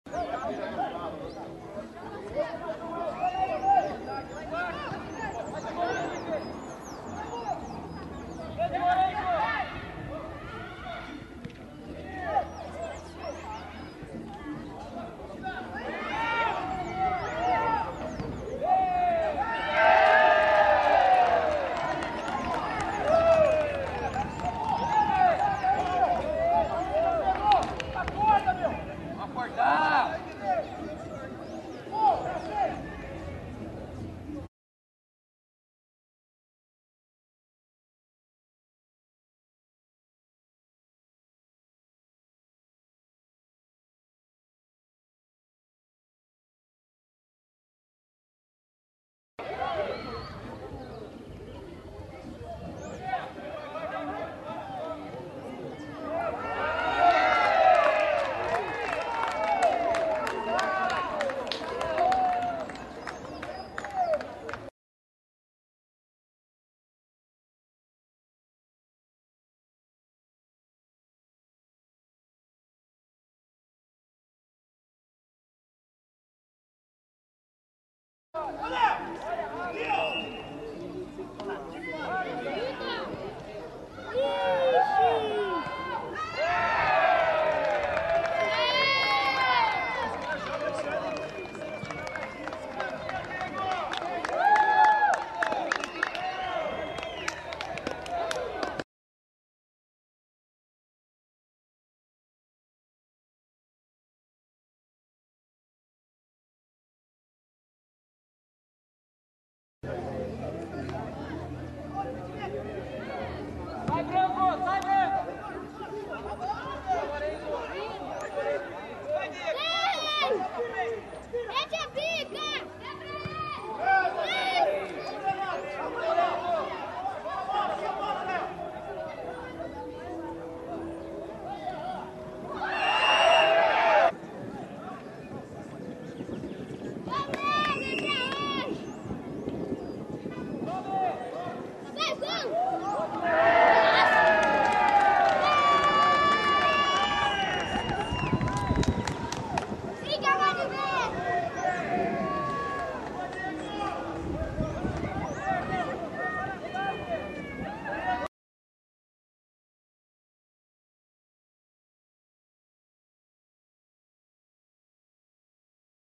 CAV X PINHEIRAO SUB-11 out2014
Gols do jogo entre CAV e Pinheirão na categoria SUB-11 pelo Campeonato Valinhense de Futebol. CAV 4 x 0 Pinhierão, com gols de Luizinho, Felipe e Diego (2).